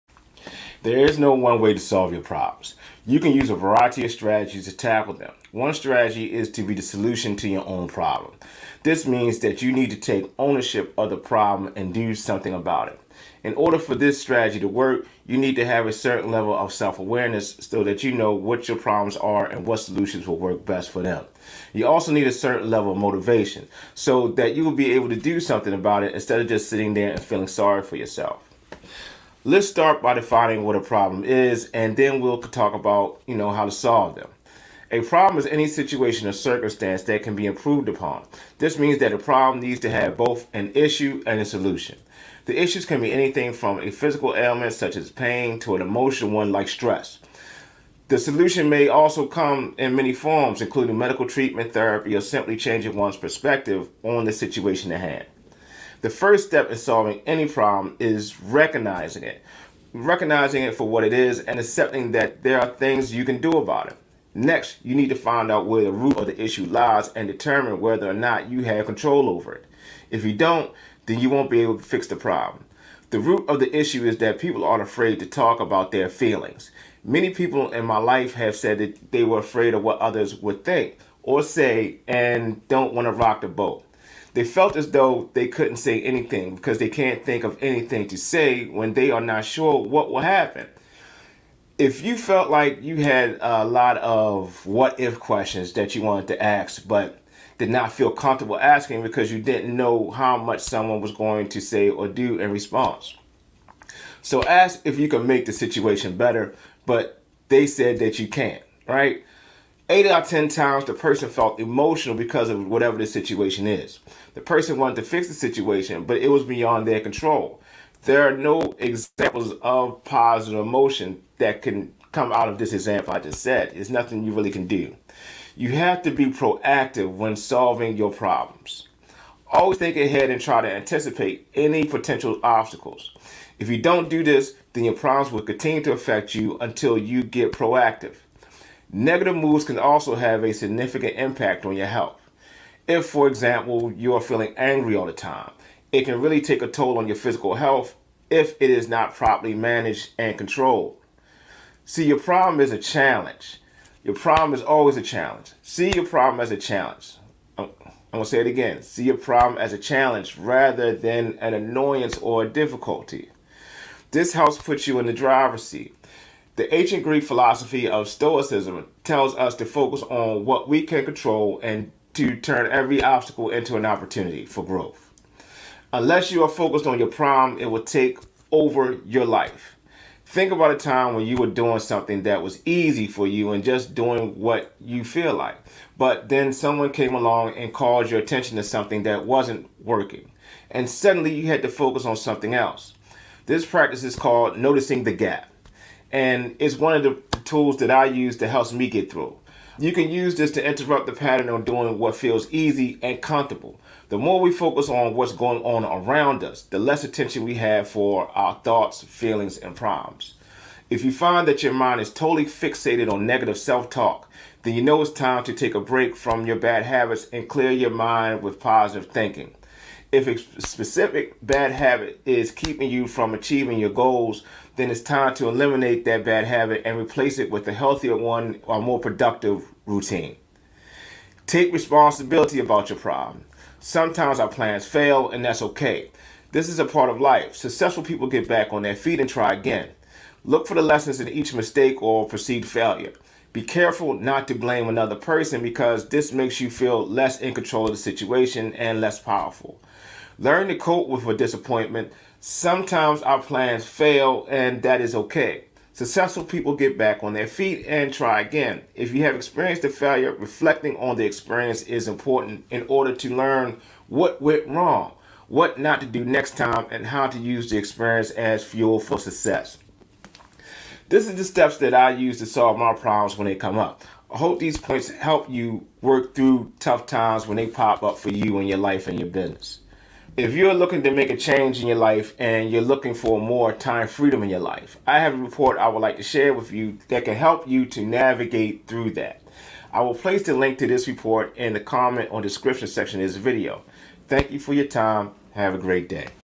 Own It: Transform Problems Into Solutions
Ready to transform your problems into powerful solutions? 🚀 Dive deep with me as we explore strategies to own your challenges and turn them into opportunities for growth. Whether you're just starting out or looking to up your online game, this video is a treasure trove of insights!

In a world where issues can range from physical ailments to emotional stress, it's crucial to have the self-awareness to identify what truly ails us and the motivation to seek out meaningful solutions. Let's have a discussion about the importance of recognizing problems, understanding their roots, and taking proactive steps to address them.

By seeing your problem as a challenge, you embrace the ancient Greek philosophy of Stoicism, focusing on what you can control and transforming obstacles into opportunities. This mindset shift is vital for both personal and business success. Remember, successful people don't shy away from failure; they learn from it and try again.

In this video, I share the practical steps I use to solve my problems, hoping they'll aid you through tough times in life and business. The link to this transformative report will be in the comment section!

Now, let's start turning those problems into solutions together. Have a great day, and remember: your problem is a challenge waiting to be conquered! 💪

CHAPTERS:
0:00 - Problem Solving Introduction
0:29 - Defining Problems
2:05 - Proactive Mindset Tips
2:31 - Challenges vs. Problems
3:51 - Responsibility in Problem Solving
4:30 - 5-Step Problem Solving Guide
4:40 - Achieving Time Freedom